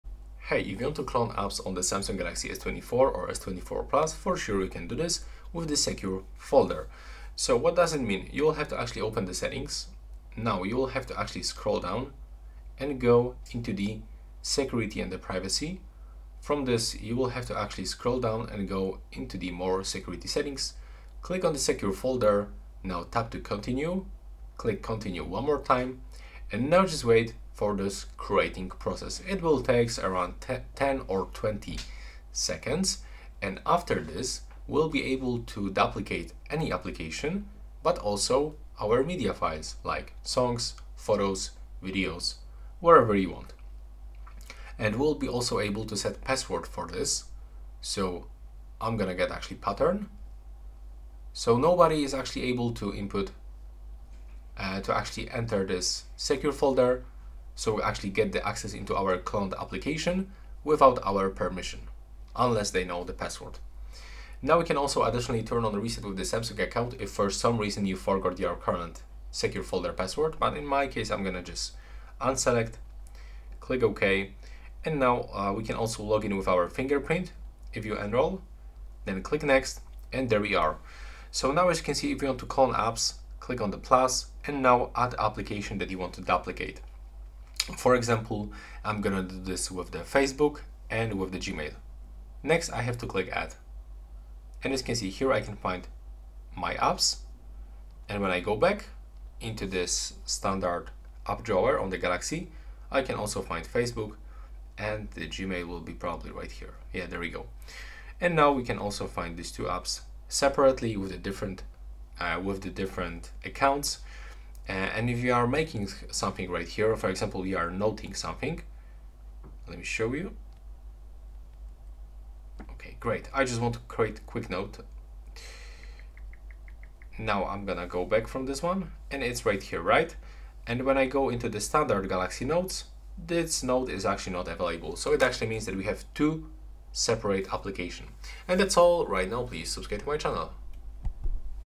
How to Clone Apps in Samsung Galaxy S24 / S24+
Discover how to clone apps on your Samsung Galaxy S24 or S24+ with our step-by-step guide. Learn the steps to use the device's built-in app cloning feature, allowing you to create duplicate copies of apps for separate accounts or purposes. Master this feature to efficiently manage multiple accounts or use multiple instances of the same app on your device.

samsung s24 clone apps
app cloner galaxy s24 plus
s24 duplicate applications
samsung s24 2 whatsapp accounts